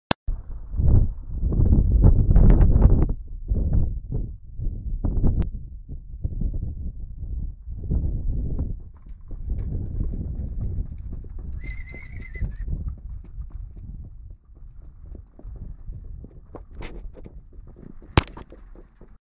Halle Berry hiitillä.wmv
Halle Berry, i Lass Zefyr, e Fiji Love, ei Spotlite Lobell. Valmentaja Janne Ranta-Krenkku, Kokemäki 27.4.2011.